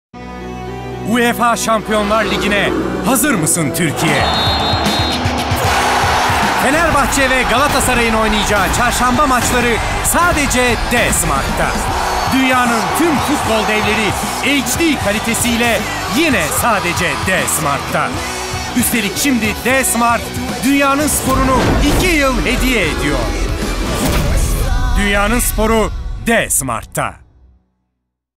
Fenerbahçe ve Galatasaray Çarşamba Maçları D-Smart'ta
D-Smart'ta her çarşamba UEFA Şampiyonlar Ligi heyecanı yaşanıyor, HD kalitesi
futbol keyfini ikiye katlıyor.